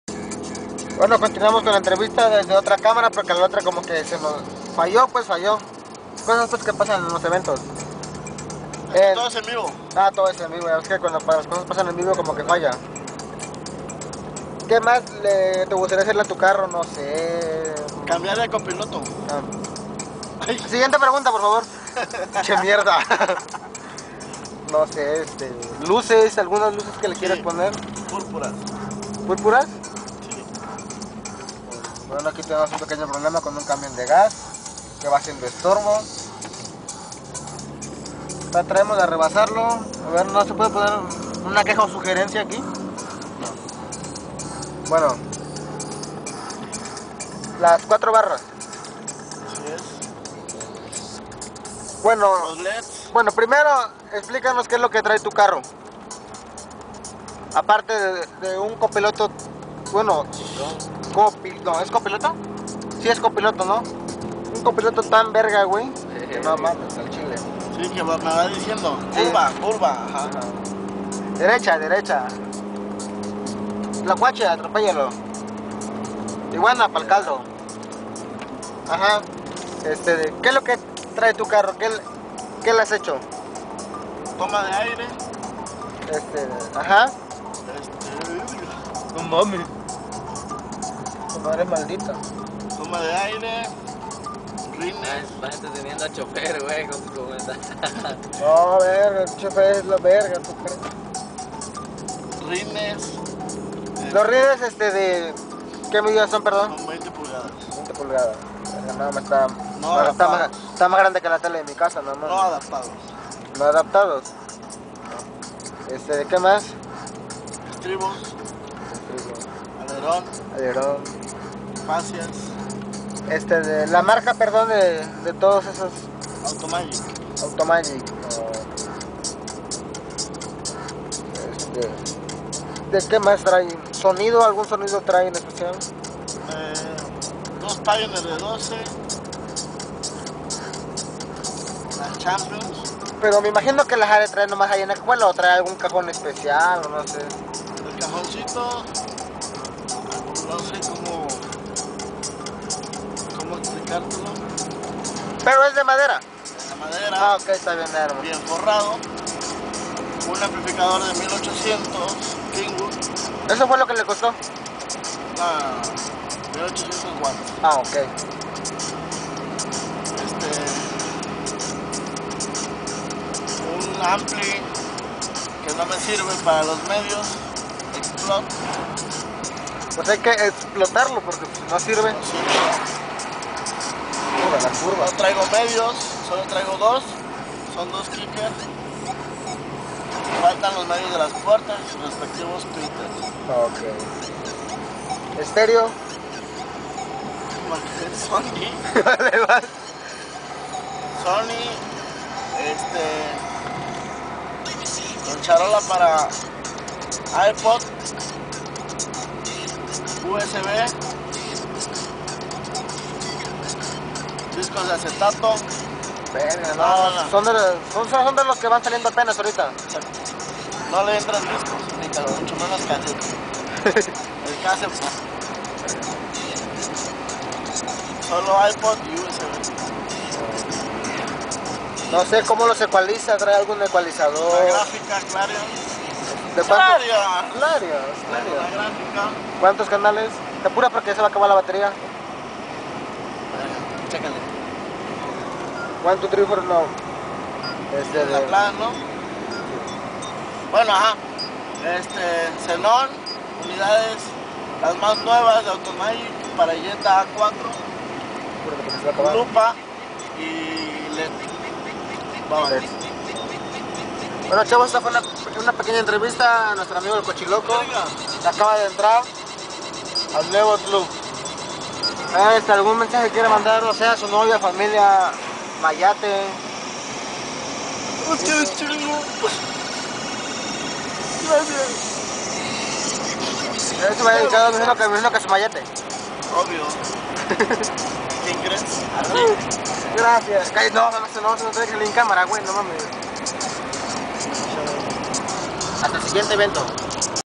entrevista a cochiloco c.r.t
buenoesta es la segunda parte dela entrevista al cochilo jaja puro desmadre veniamos diciendo  xplicaba de lo qe traia su carro i lo qe le va poner xido cochi trajisre trofeo